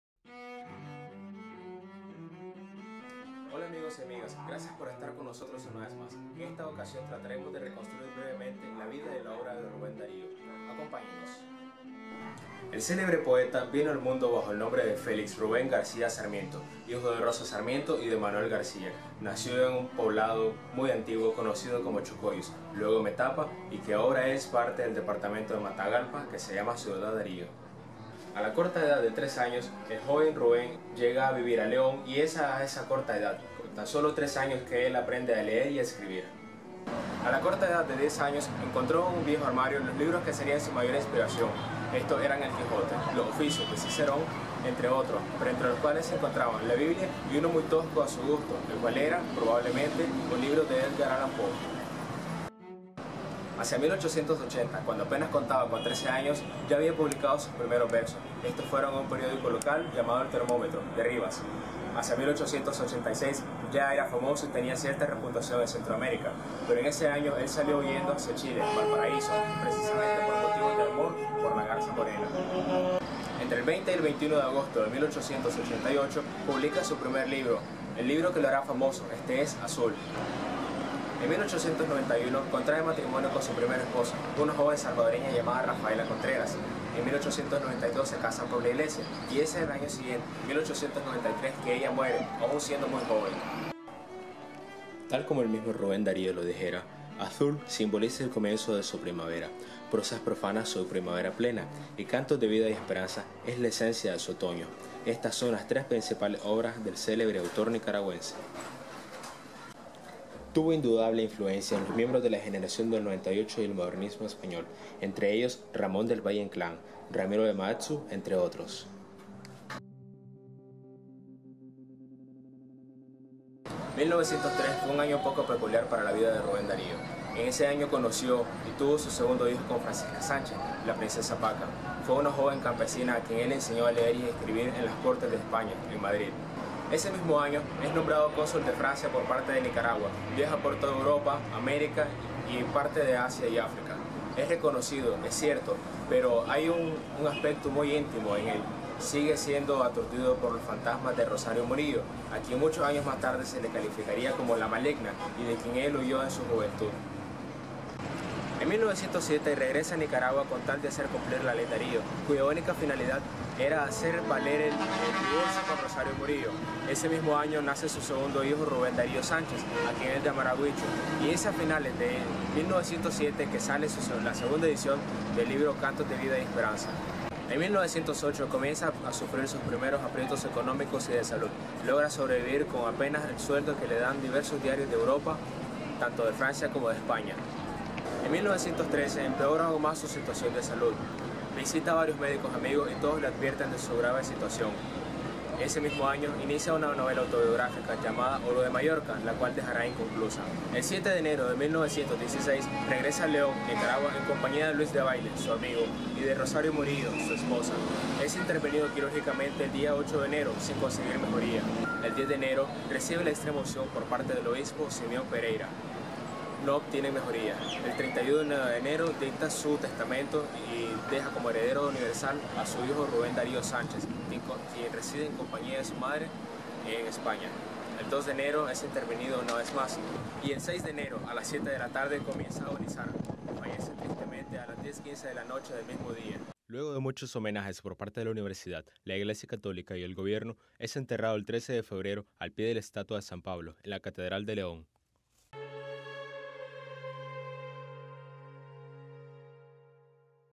Reportaje Rubén Darío 1
Vida de Rubén Darío en fotos, narrada por el estudiante de Comunicación y miembro del equipo de Nuevas Miradas, Carlos Morales Zapata